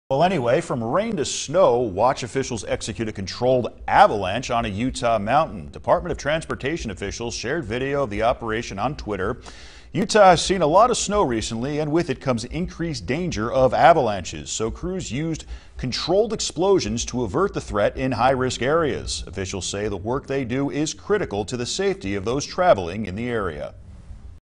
Utah Mountain Controlled Avalanche
Watch officials execute a controlled avalanche on a Utah mountain.